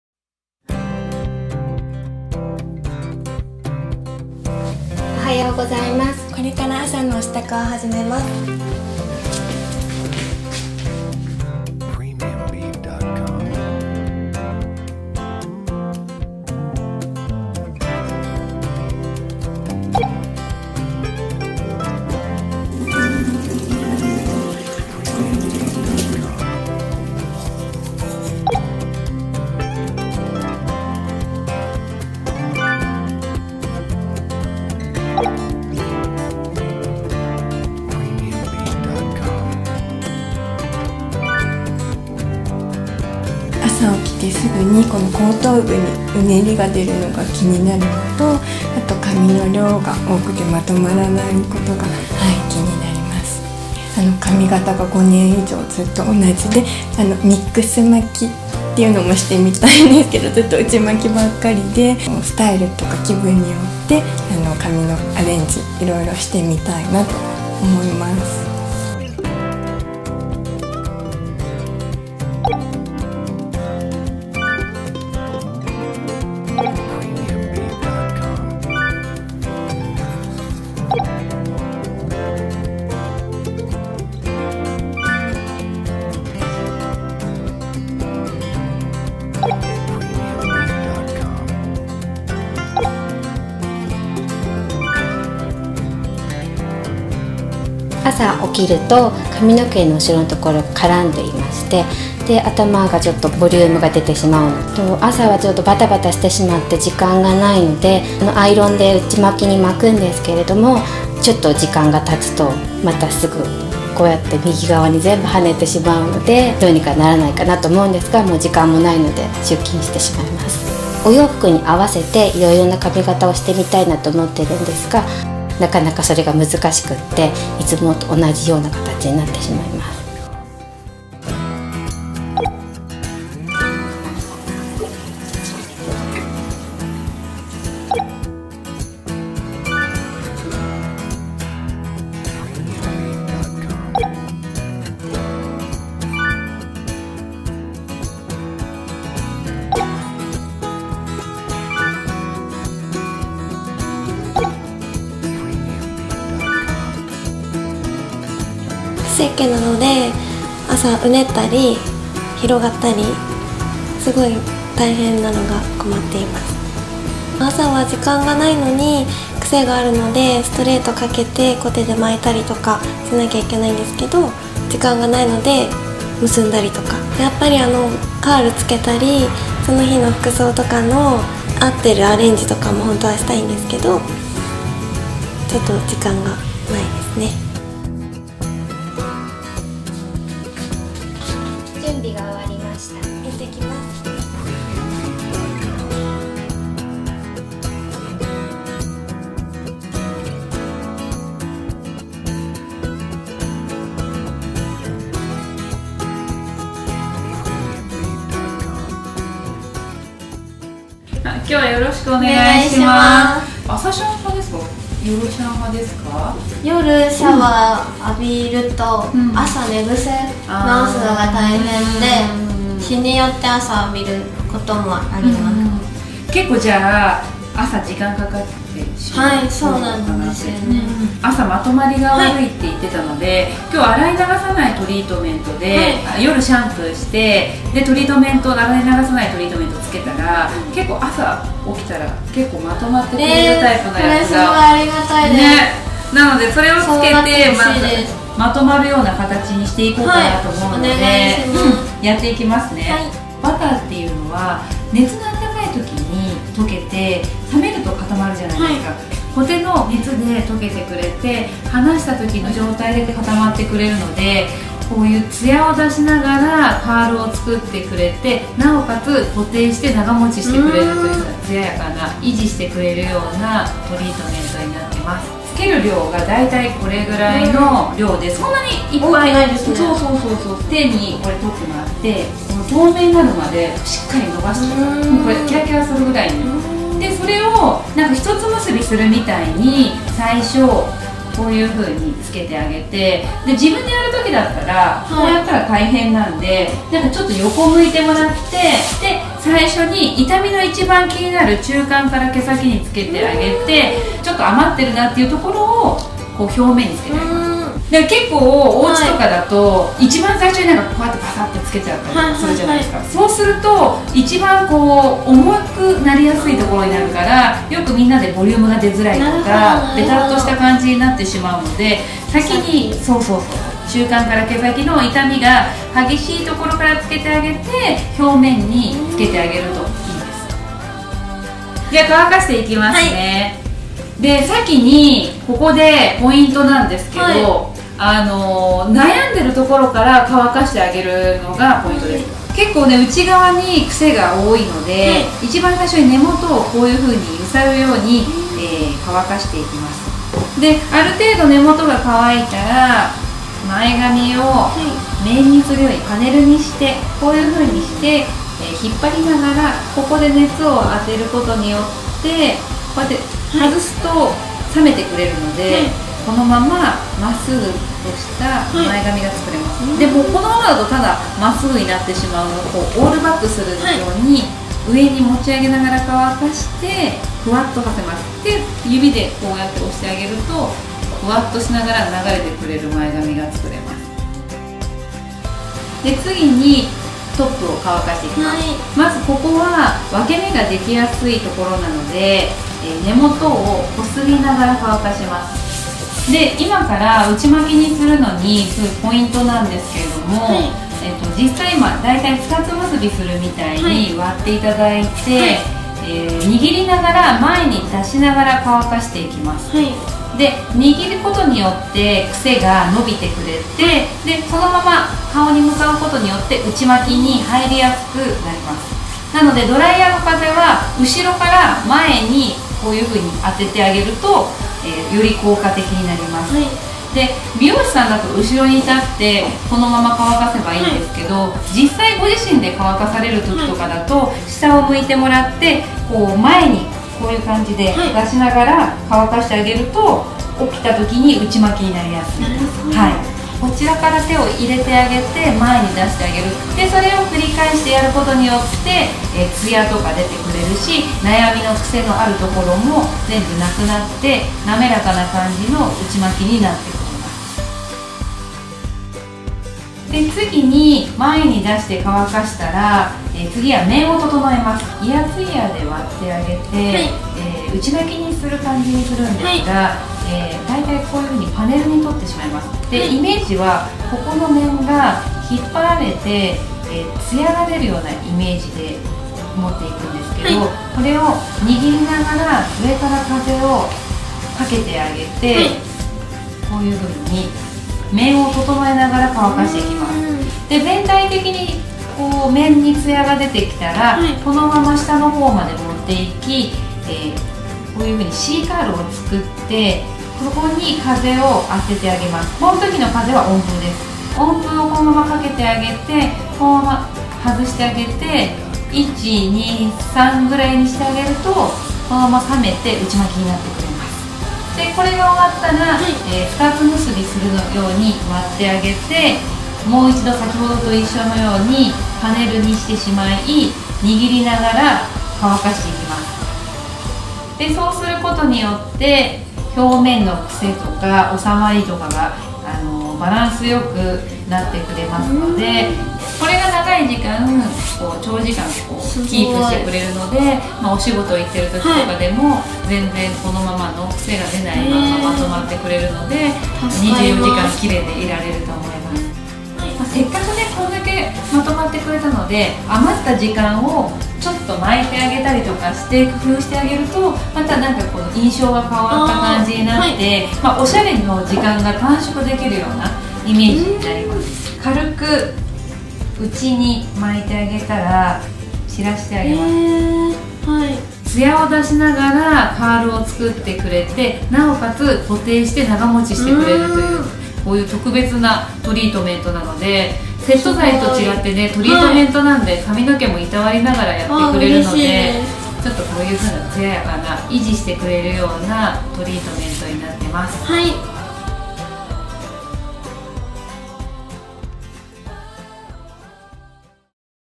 ジェミールフランメルティバター『顧客のお手入れ事情＆カウンセリングＶＴＲ』
夜はしっかり乾かしてまとまっていたはずの髪が、朝起きたら広がってる…。時間のない朝は寝ぐせ直しで精いっぱい。ほんとは服に合わせてコテで巻いたりアレンジしたりしたいのに。。。そんな忙しい朝の女性のリアルをぜひご覧ください。
後半は、デザイナーさま向けに、サロンでのメルティバターを使った20代女子の心を掴んで離さないコミュニケーションの方法を収録。